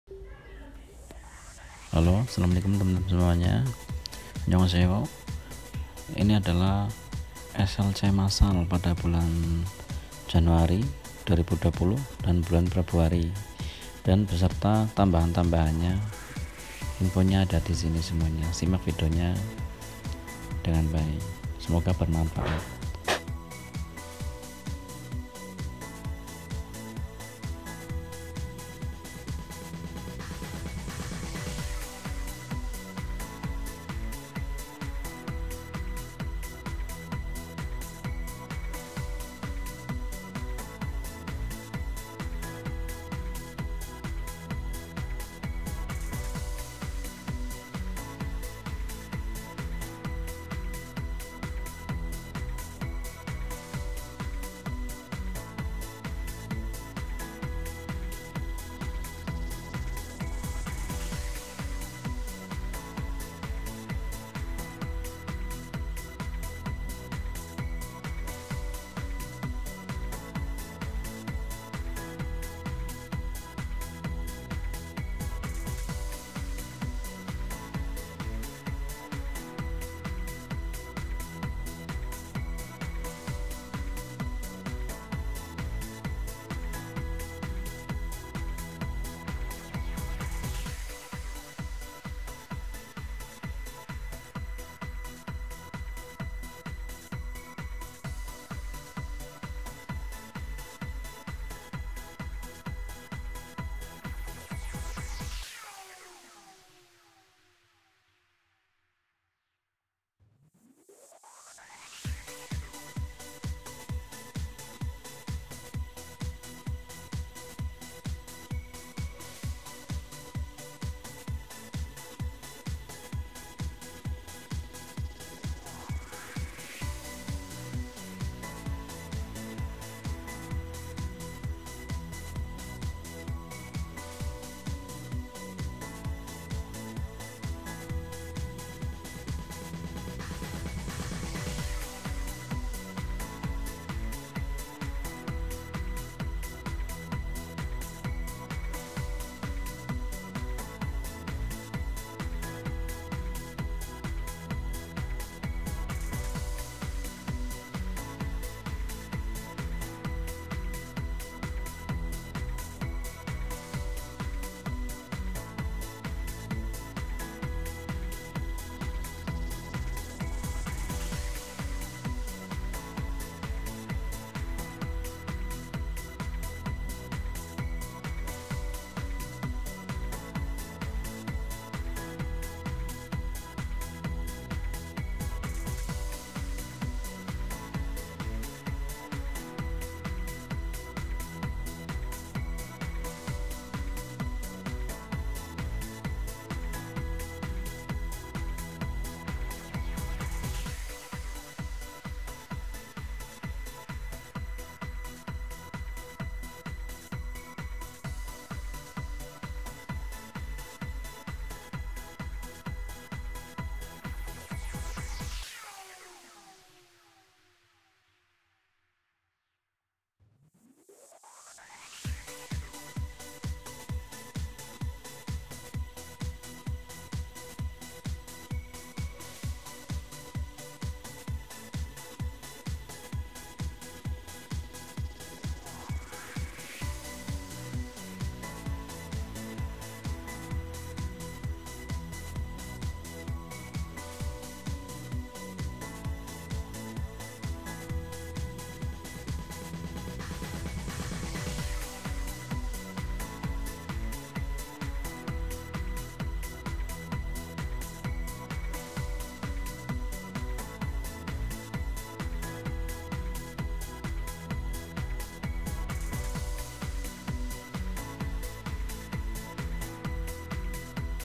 slcmasal Slc masal korea selatan january-february 2020 dan kuota tambahannya
Membahas kisi” ujian dari bab 1-60 persiapan ujian cbt korea